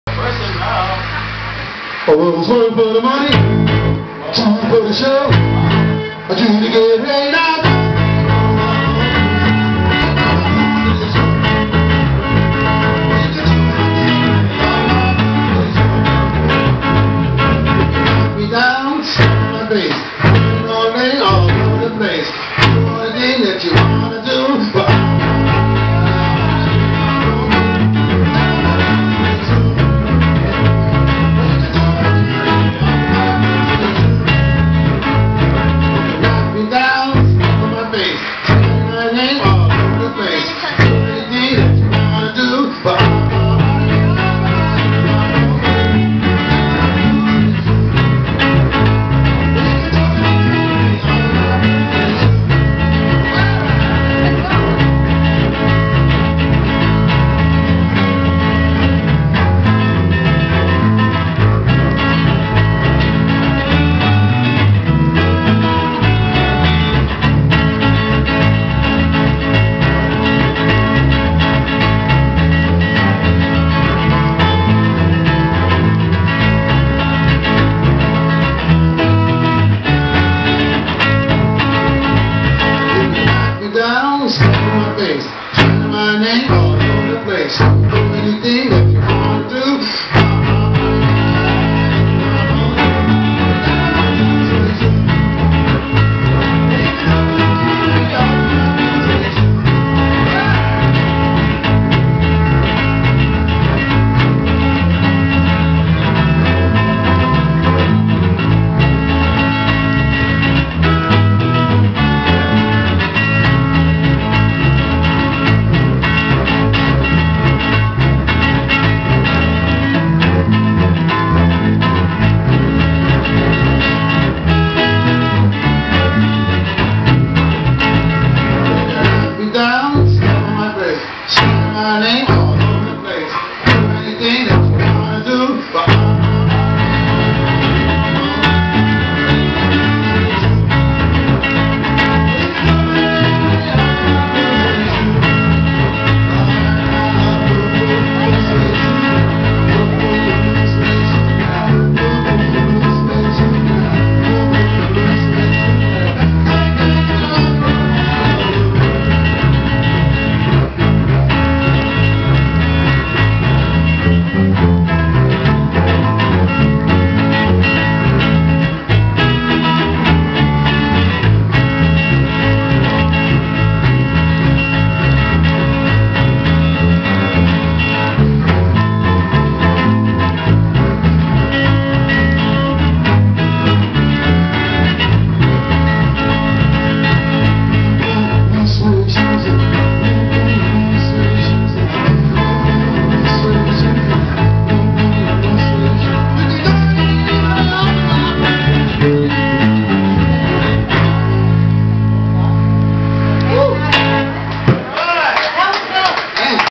Cover of Elvis Presley
My dad and uncle playing some Elvis Presley